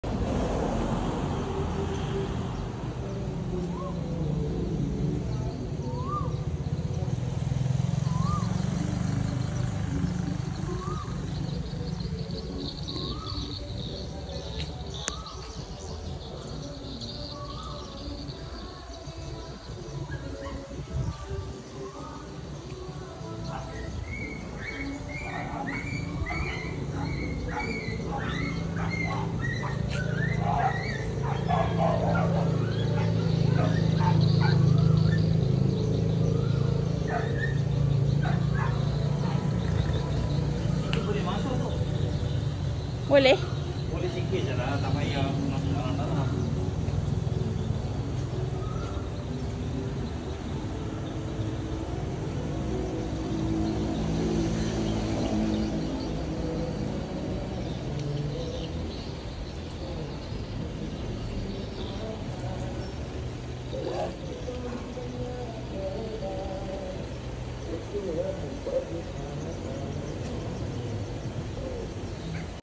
Fishing boats Jetty at Sekinchan.
Nice view at fishing boats jetty.
Located at Sekinchan, Kuala Selangor